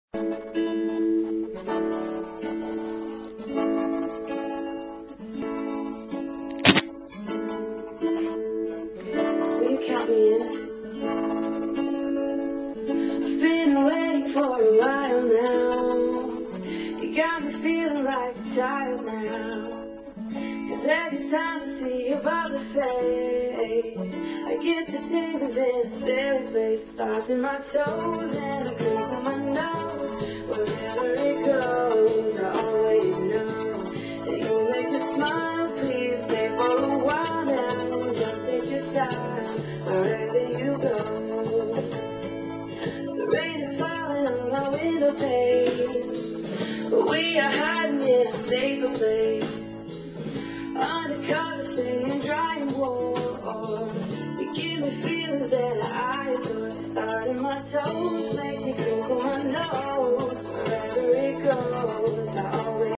Bubbly Phoebe
Recorded on December 25, 2008 using a Flip Video camcorder.My Cavalier King
Charles Spainel.